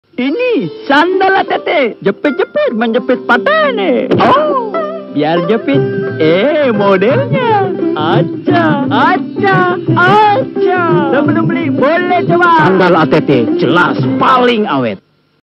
Iklan Sandal ATT - Jelas Paling Awet (1997) @ RCTI, Indosiar, TPI, ANteve, & SCTV
Iklan ini pernah ditayangkan di RCTI, Indosiar, TPI, ANteve, & SCTV pada tahun 1997.